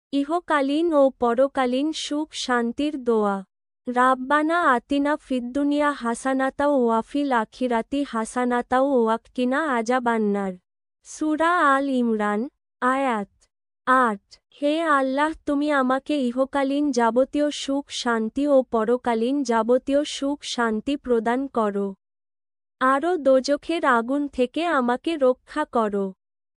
ইহকালীন ও পরকালীন সুখ-শান্তির দোয়া
ইহকালীন ও পরকালীন সুখ-শান্তির দোয়া "রাব্বানা আতিনা ফিদ্দুনিয়া হাছানাতাঁও ওয়াফিল আখিরাতি হাছানাতাঁও ওয়াক্বিনা আজাবান্নার"
[অর্থঃ হে আল্লাহ্ তুমি আমাকে ইহকালীন যাবতীয় সুখ-শান্তি ও পরকালীন যাবতীয় সুখ-শান্তি প্রদান কর। আর দোজখের আগুন থেকে আমাকে রক্ষা কর।]